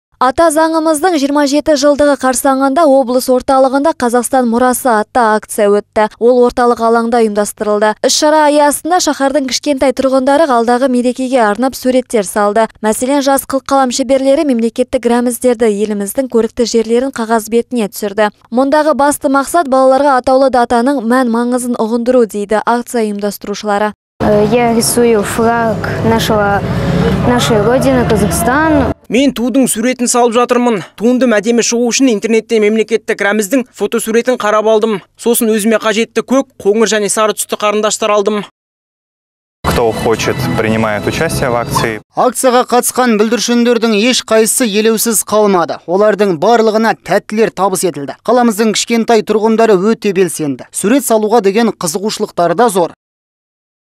«Қазақстан мұрасы» акциясы
Ата заңымыздың 27 жылдығы қарсаңында облыс орталығында «Қазақстан мұрасы» атты акция өтті. Ол орталық алаңда ұйымдастырылды. Іс-шара аясында шаһардың кішкентай тұрғындары алдағы мерекеге арнап суреттер салды. Мәселен, жас қылқалам шеберлері мемлекеттік рәміздерді, еліміздің көрікті жерлерін қағаз бетіне түсірді. "Мұндағы басты мақсат - балаларға атаулы датаның мән-маңызын ұғындыру",- дейді акция ұйымдастырушылары.